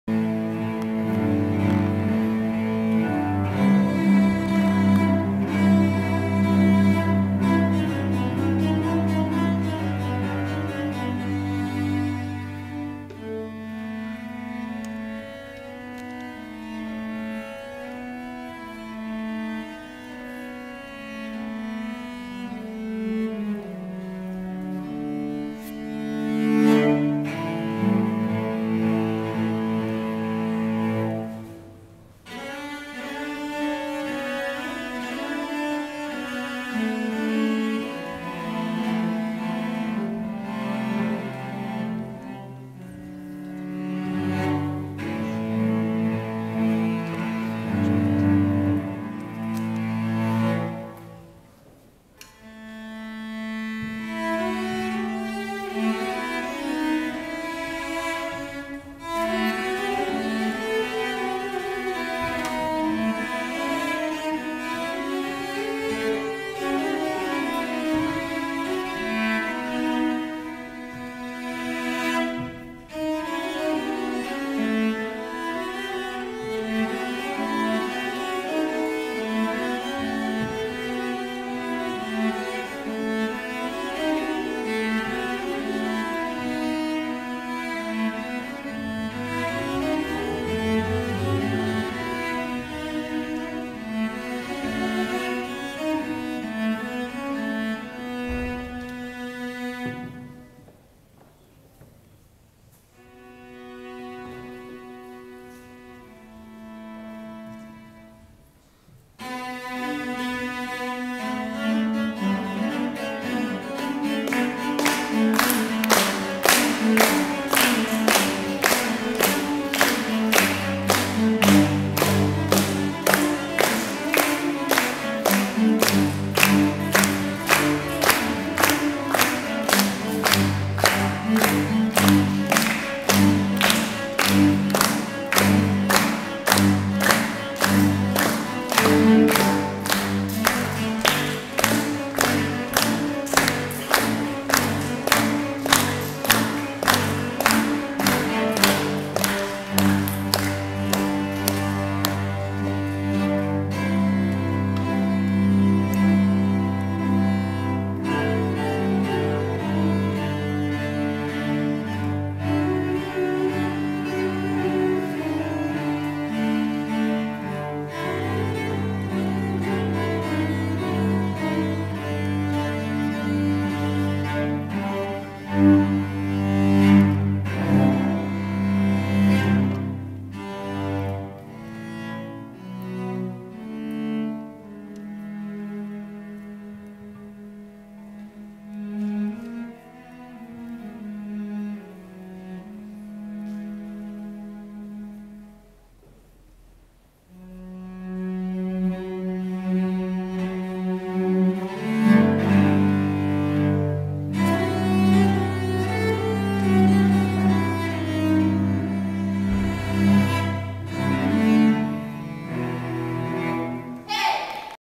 duet wiolonczelowy.AVI
Koncert w ramach kursu muzycznego WAKACJE Z NUTKAMI 2009